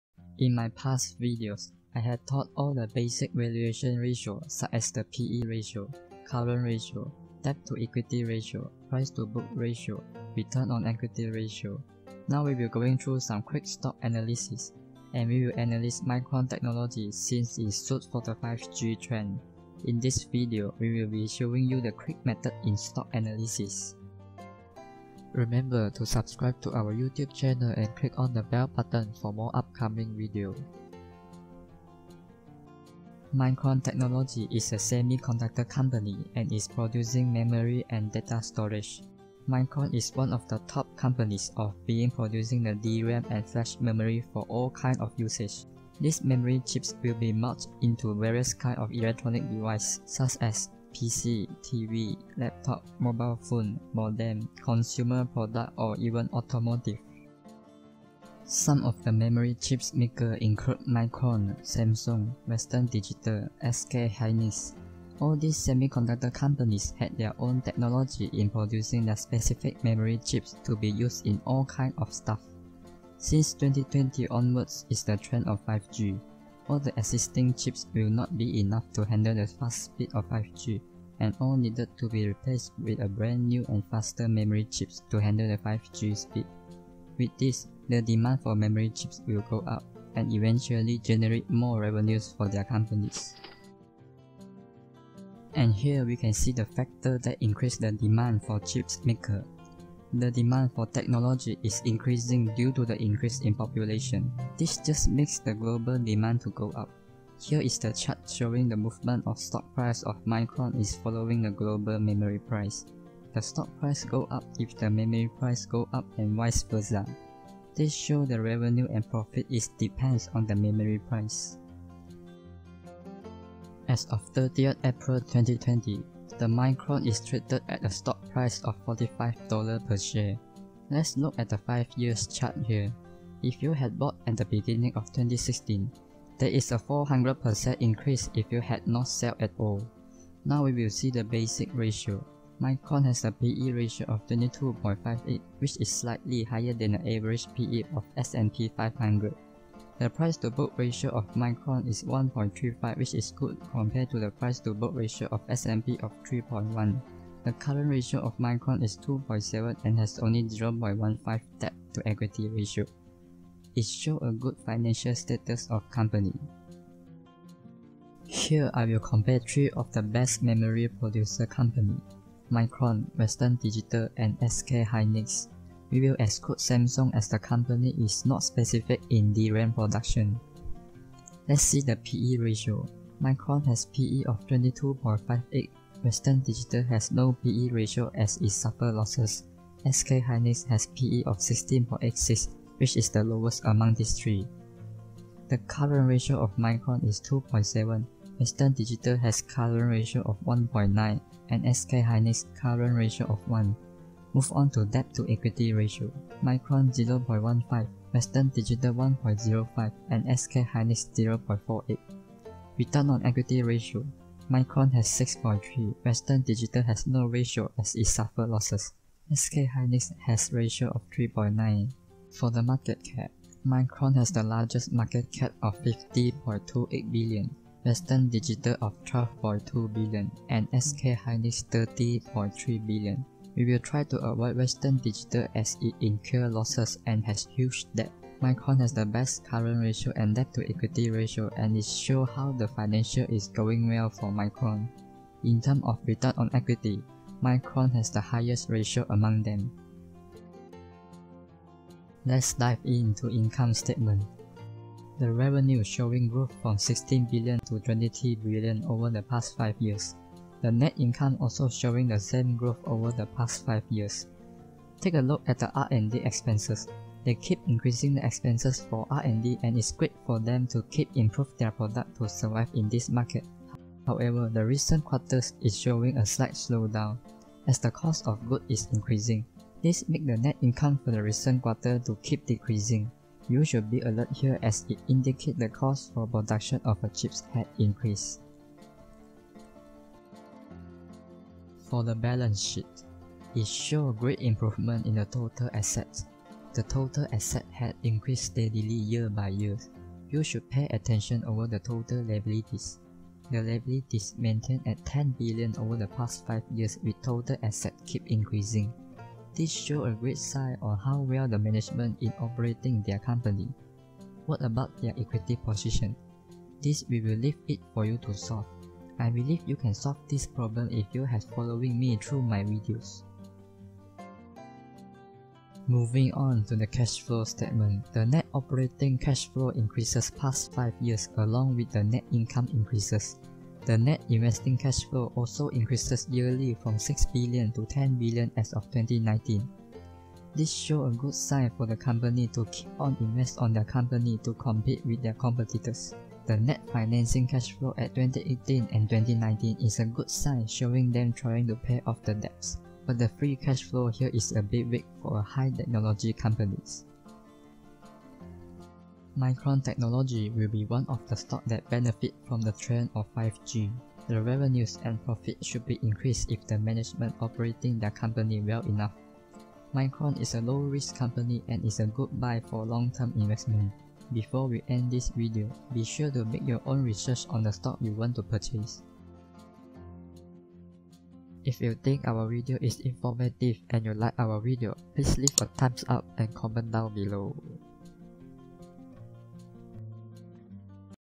Analyze Stock Quick | Interpretation of Financial Statements | Is Micron a Buy
2020 on wards is the trend of 5G an will bring another decade of tech stock to grow up. Try to anticipate a good technology stock that will benefit from the 5G and it will make your investment success.

Micron Technology, Inc. is an American producer of computer memory and computer data storage including dynamic random-access memory, flash memory, and USB flash drives. It is headquartered in Boise, Idaho. Its consumer products are marketed under the brands Crucial and Ballistix.

This video show you on how to do a quick stock analysis based on all the basic investment terms. We look at the valuation ratio and financial ratio of the company and compare with its competitors or peers to determine their future prospect. We also look deep into the annual reports or the financial reports to determine how well the management in operating their business.
Whether its has huge debt or low debt, whether they reinvest in their company or they pay out dividends.

Stock analysis is the method used by a trader or investor to examine and evaluate the stock market. It is then used to make informed decisions about buying and selling shares. ... Stock analysis can be used to gain an insight in to the economy as a whole, the stock market, a specific sector or an individual stock.

In this video, we will analyse on Micron Technology (MU) which has showed a great financial status of their company. Micron technology is producing all kind of memory chips and mostly DRAM and Flash memory.
DO your research before entering the stock market.
Prepare cash in hand to handle the coming bear market (recessions)